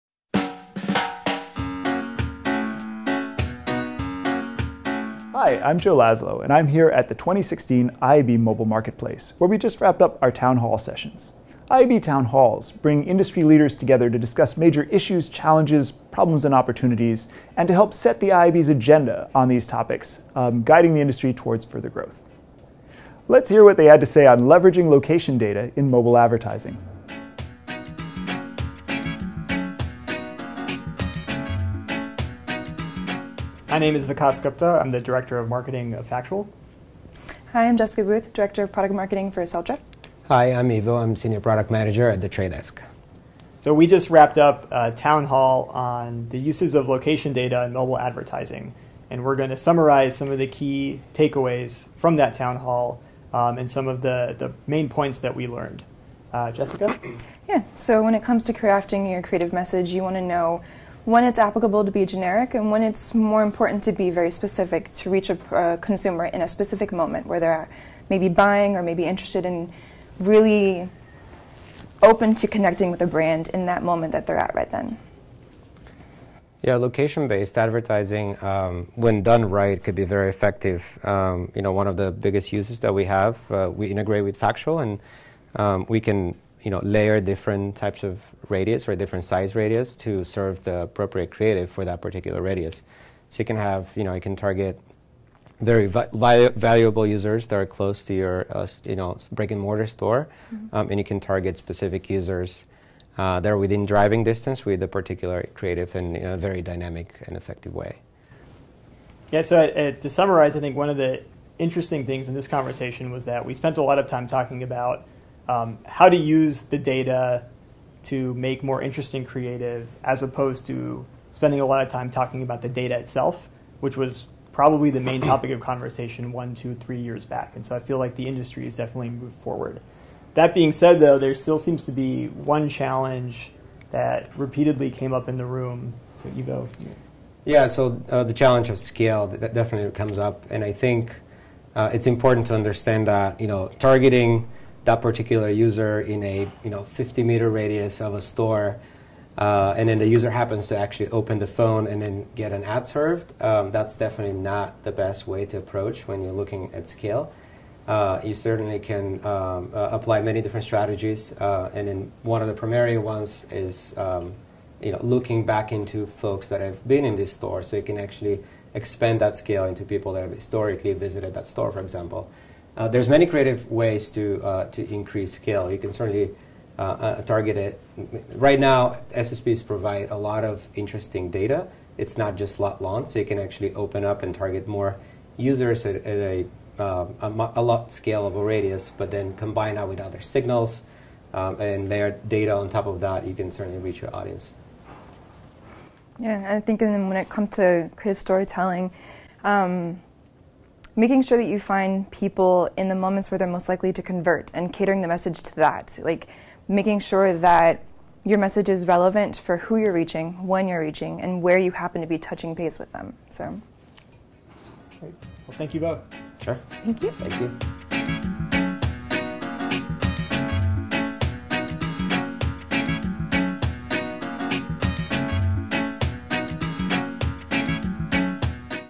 2016 IAB Mobile Marketplace: Town Hall Recaps
Learn what took place during the closed-door Town Halls at the 2016 IAB Mobile Marketplace. Hear from Vikas Gupta, Factual,  Ivo Karadjov, The Trade Desk, and Jessica Booth, Celtra, on Leveraging Location Data in Mobile Advertising.